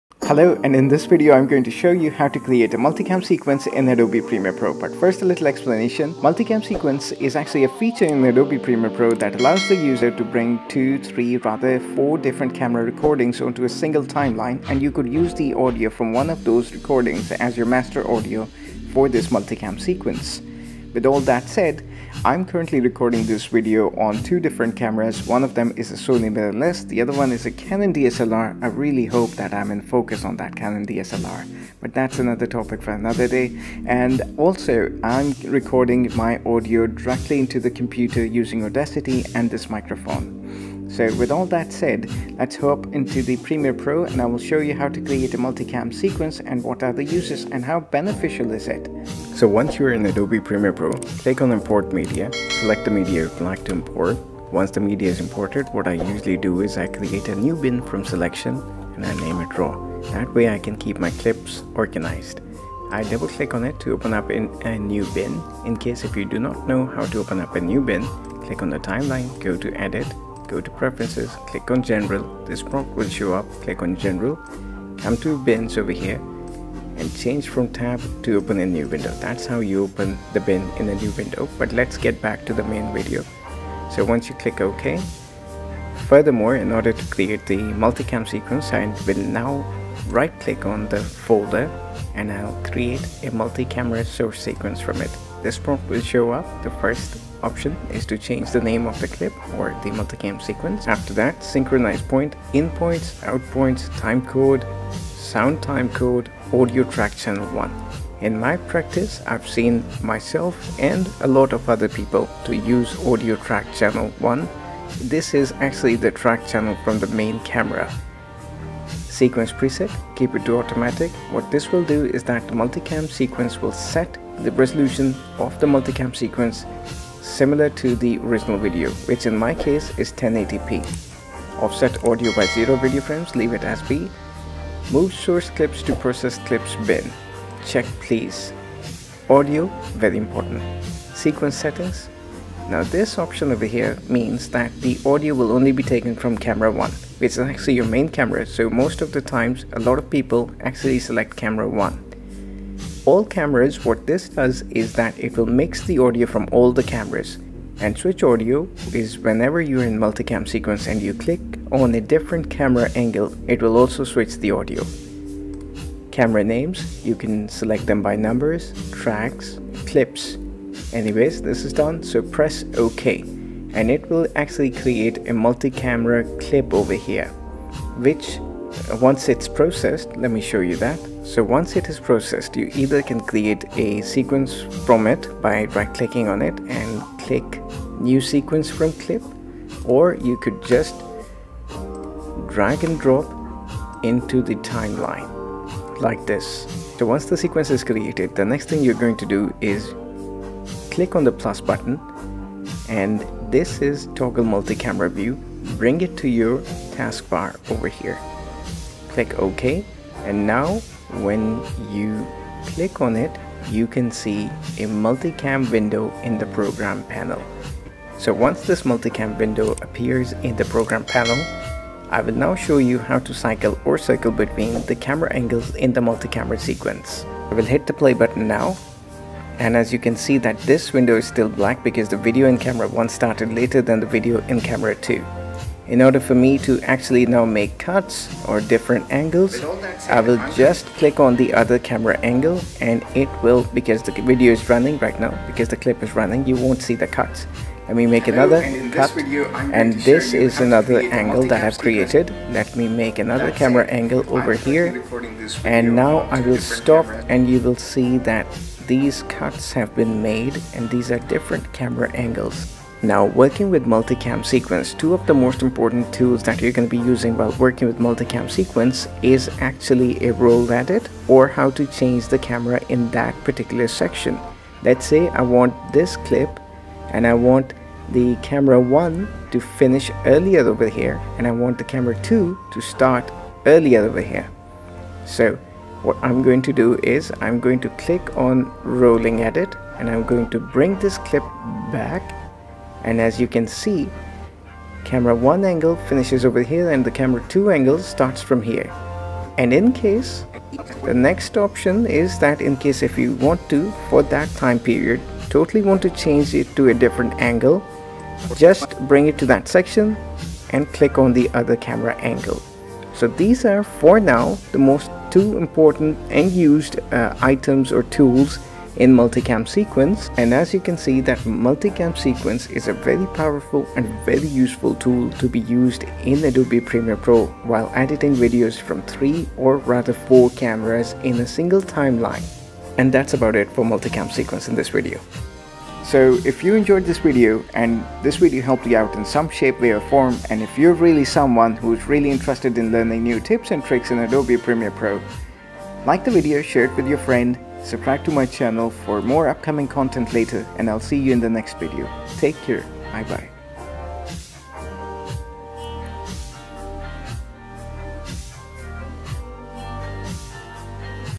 Multi-Camera Editing in Adobe Premiere Pro CC 2020 tutorial - FAST
How to Create a Multicam Sequence with Audio Sync in Premiere Pro | Adobe Creative Cloud

In this tutorial  I want to shows you how to set-up and create a Multicam sequence, leveraging Premiere Pro's powerful audio-sync feature.